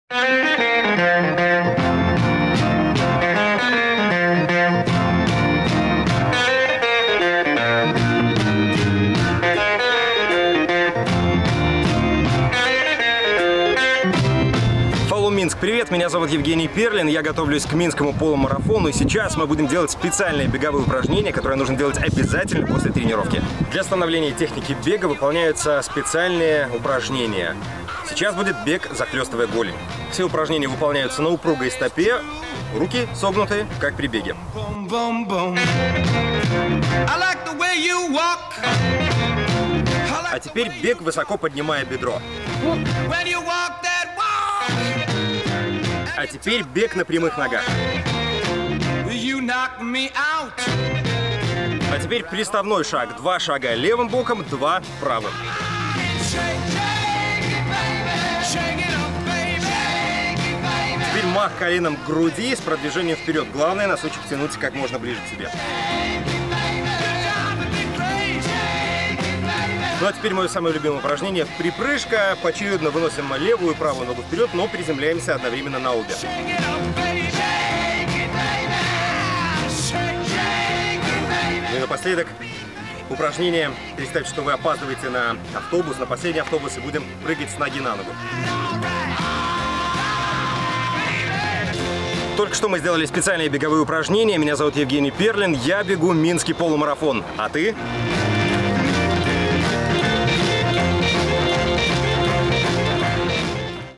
Специальные беговые упражнения с телеведущим Евгением Перлиным
Беги вместе с нами! Follow Minsk! Follow